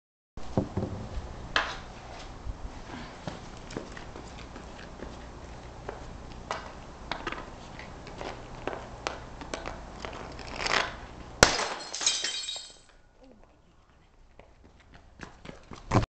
Broken Glass Hockey Shot
This should be on America's funniest home videos. I really truely did break the window in my garage.